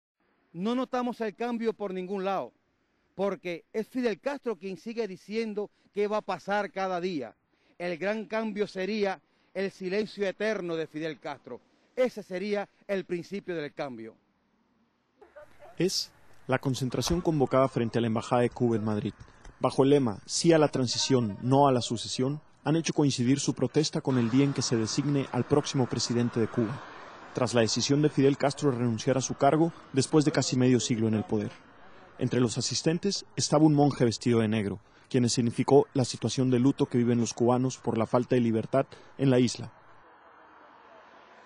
Disidentes cubanos: sólo con "silencio eterno" llegará democracia
Madrid (España), 24 feb (EFE).-La plataforma "Cuba, Democracia Ya" advirtió que sólo con el "silencio eterno" de Fidel Castro llegará la democracia a la isla, durante la concentración que convocó frente la embajada de Cuba en Madrid.
Bajo el lema, "Sí a la transición, no a la sucesión", este colectivo disidente hizo coincidir su protesta con el día en que está previsto que se designe al próximo presidente de Cuba, tras la decisión de Fidel Castro de renunciar a su cargo después de casi medio siglo en el poder.
Entre los asistentes a este acto un monje vestido de negro escenificó la situación de luto que viven los cubanos por la "falta de libertad" en su país.

Palabras clave: cuba-sucesion-fidel-castro-disidentes-embajada-transicion.